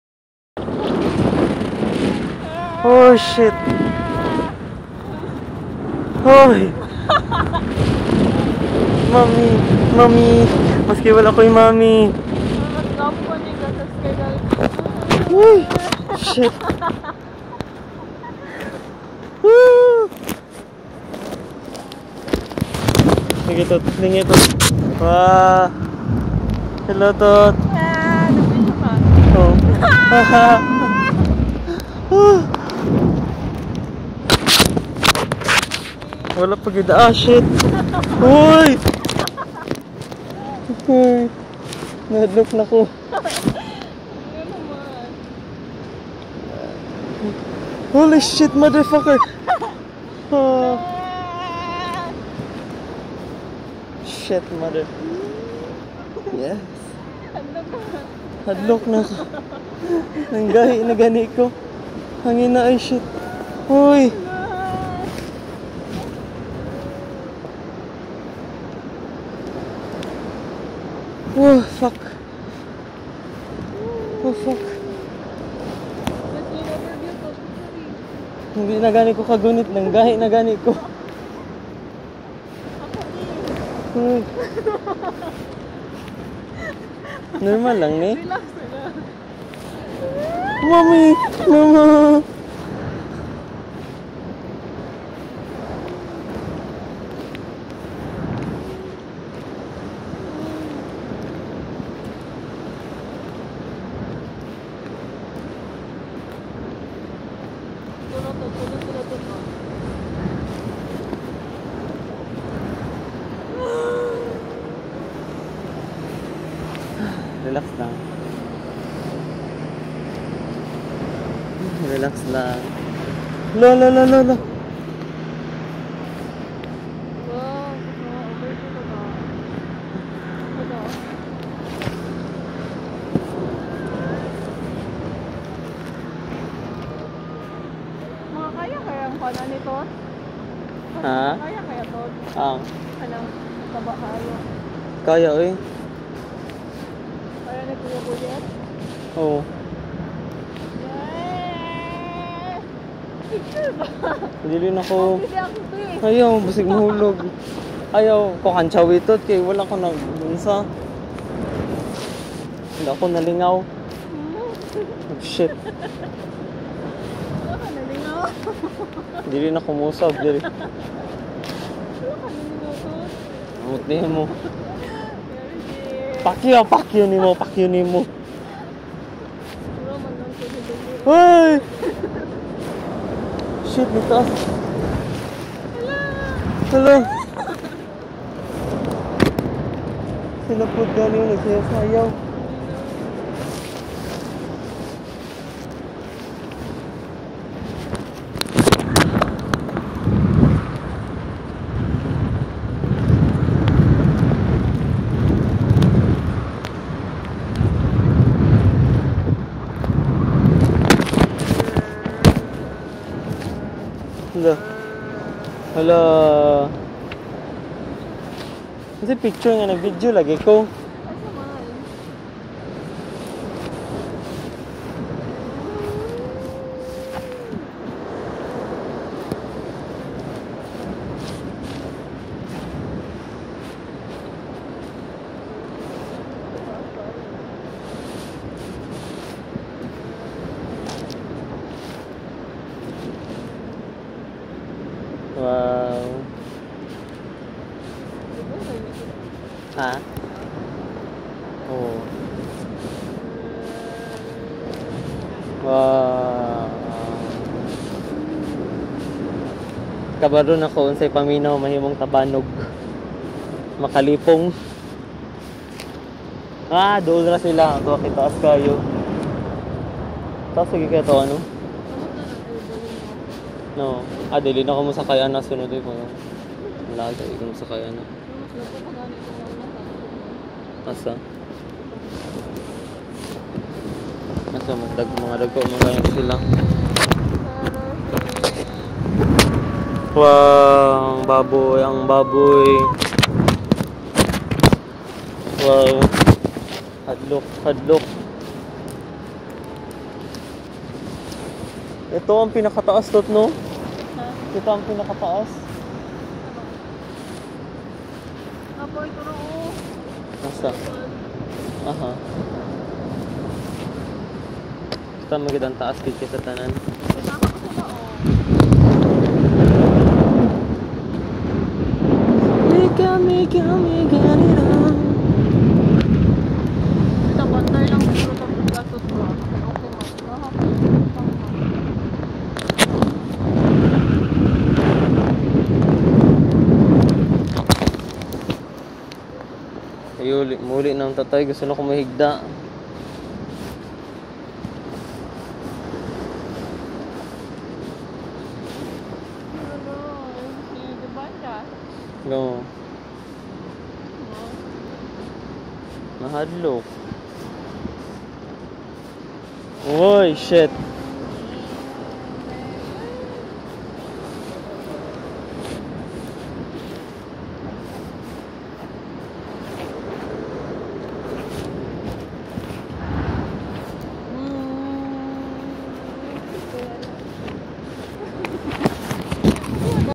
Parasailing at Boracay Island
Overview of Boracay Island. Sorry for the bad mouth. Im really scared of heights. :)